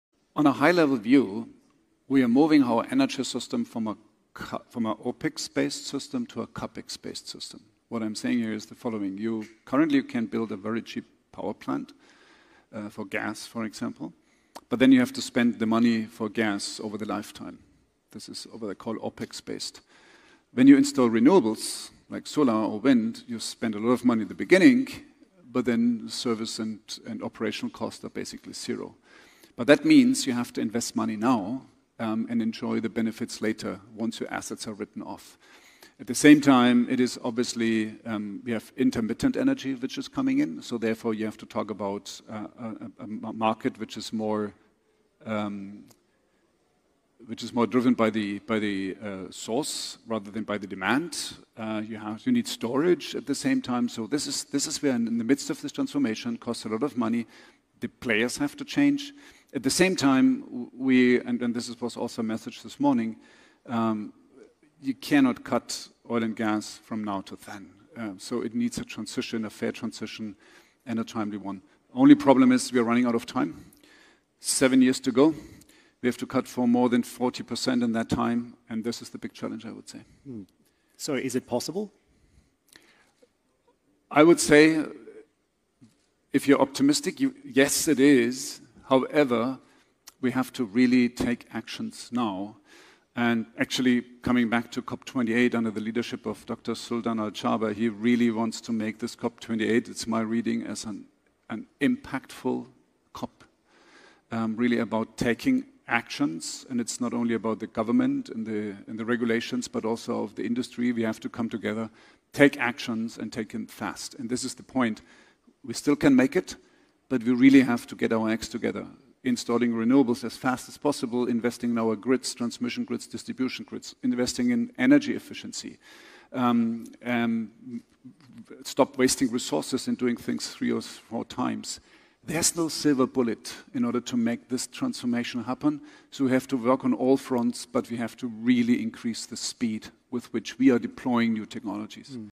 'We are running out of time,' Siemens CEO says on energy transition
Roland Busch, Siemens CEO, says we are running out of time," on the energy transition. "We have to really take action now," he says.